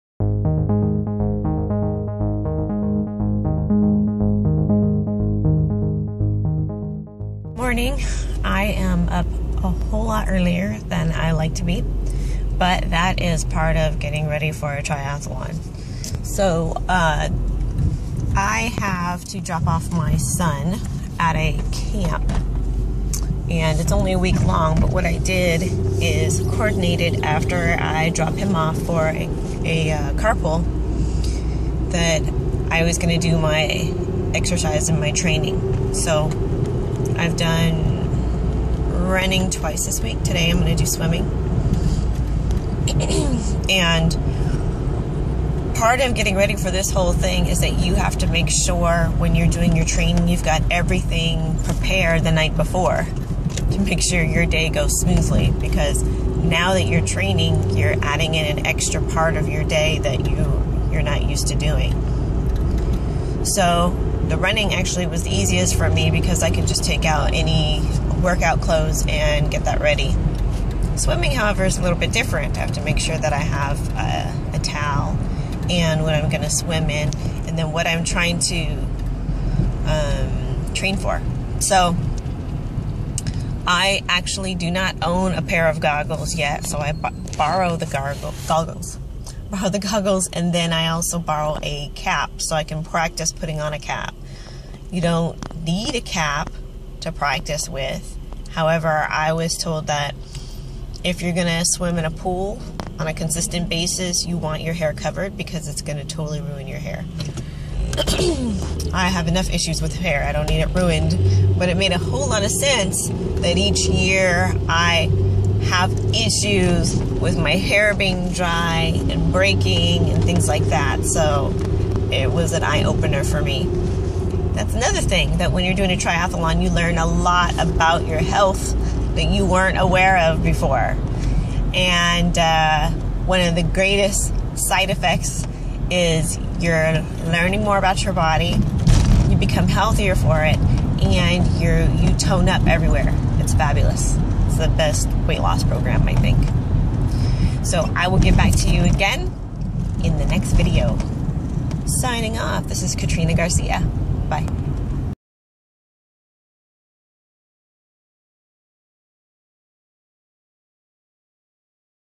Tri training on a tight schedule
With a little over a month, I began my triathlon training. the challenge was fitting into my schedule or really creating a new schedule. My point of motivation (regarding consistency) was to actually see what the track looks like. I went with my sister to preview her recent triathlon. Viewing the ocean and biking the course ahead of time made an impact on me.